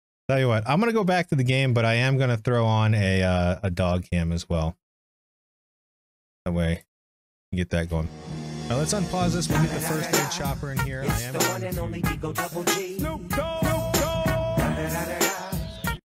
When Buddy steals a pillow because he didn't get a treat!
Buddy decides to steal a pillow before bouncing.